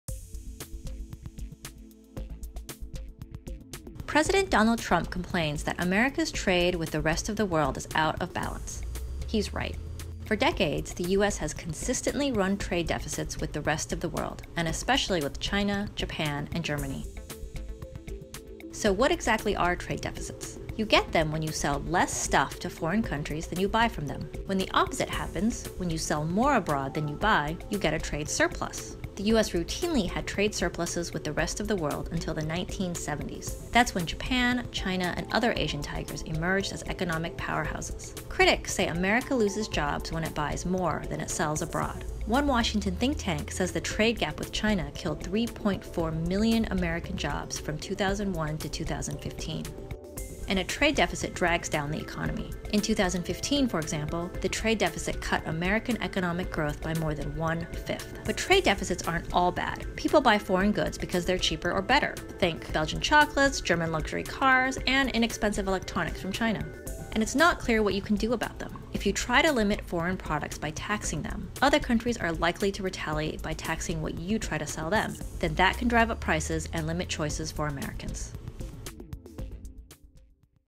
Trade deficits explained
A look at trade balance between U.S. and the rest of the world